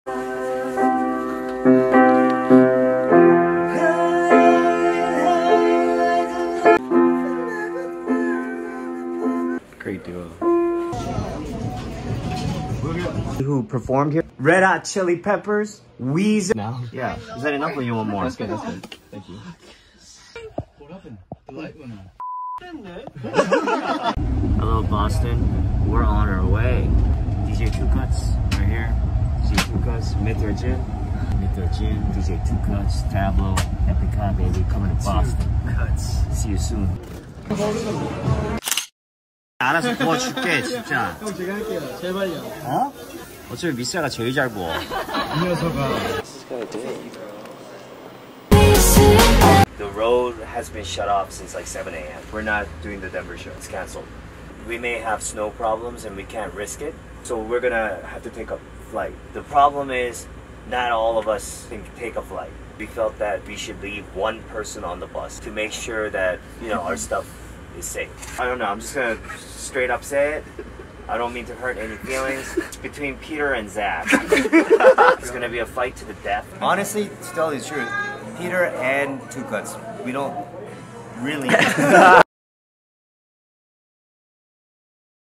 Woosung’s cut at Epik High Tour Vlog in 2022 (ft. Peter)
Woosung had been touring with Epik High last year and i just wanted to keep the memories of him being on the tour again after the hiatus. All videos and pics aren't mine, those are from @blobyblo on instagram. Enjoy!

Ps. Peter is woosung’s high school best friend